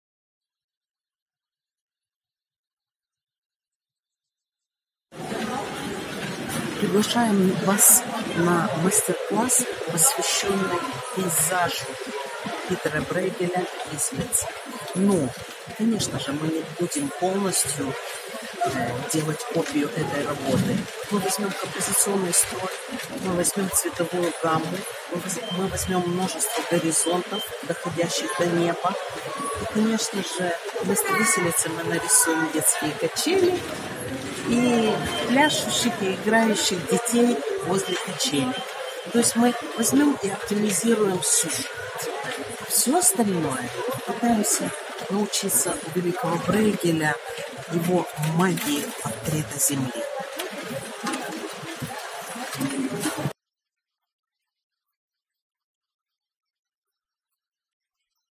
Брейгелевский живописный почерк - Зимние качели
Пространства Брейгелевских пейзажей приводят в неописуемый восторг ценителей живописи.Уходящие в даль горизонты наполнены жизнью природы – озера и реки, лесные массивы и отдельно стоящие деревья – великаны, дома деревенских жителей и церквушек – все рассматривается зорким глазом художника! На переднем плане происходит радостное событие – встреча первого снега! Дети прыгают и веселятся, играя с белыми снежками. Качели превращаются в снеговую катапульту!